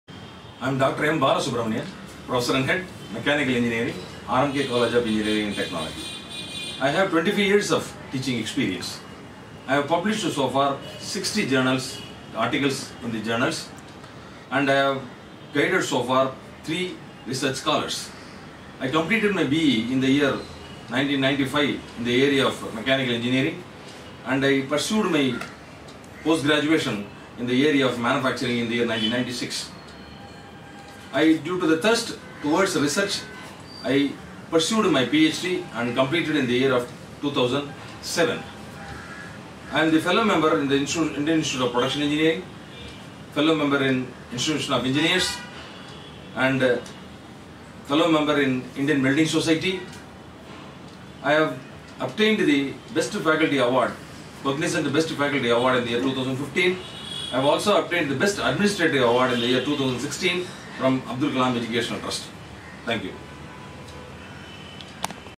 M Balasubramanian - Academic Non Circuit Faculty - ASDF International @ SIAA Awards 2019
A self-introduction of all the Finalists and Winners.

Join us on April 06, 2019 at the Hyderabad, Telangana, India to witness the event.